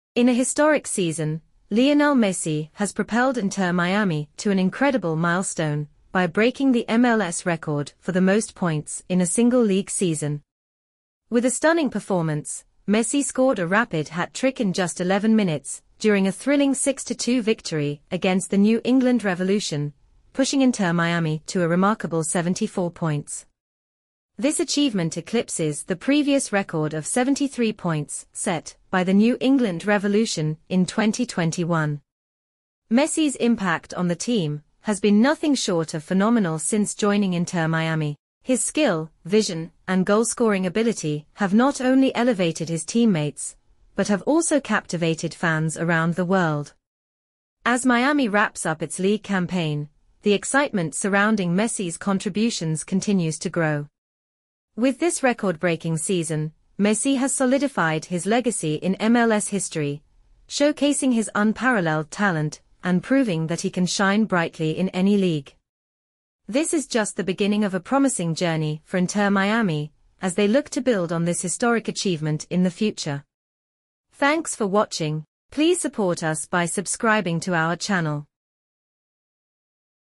Lionel Messi Breaks MLS Record: Inter Miami's Historic Season Unveiled
Lionel Messi has made history with Inter Miami, breaking the MLS record for the most points in a single season. Discover how his stunning hat trick propelled the team to 74 points, surpassing the previous record. Witness Messi's magic and Miami's incredible journey in the 2024 MLS season!